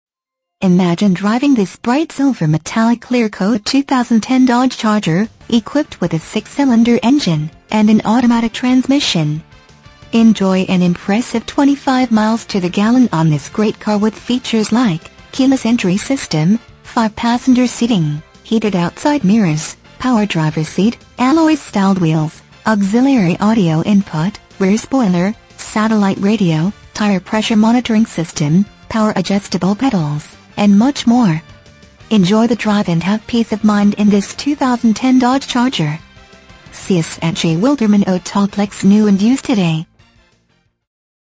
2010 Dodge Charger J Wilderman Autoplex New/Used Mt Carmel, IL 62863
Dodge Charger Check out this Bright Silver Metallic Clear Coat Dodge Charger 4 Dr Sedan, equipped with a 6 Cylinder engine and an automatic transmission. Enjoy an impressive 25 miles to the gallon on this great car with features like Keyless Entry System, 5 Passenger Seating, Heated Outside Mirrors, Power Driver's Seat, Alloy Styled Wheels, Auxiliary Audio Input, Rear Spoiler, Satellite Radio, Tire Pressure Monitoring System, Power Adjustable Pedals, and much more. Enjoy the drive and have peace of mind in this Dodge Charger. See us at J Wilderman Autoplex New/Used today!
MILES: 102,836
VIN:2B3CA3CV3AH107304

1422 West 9th St.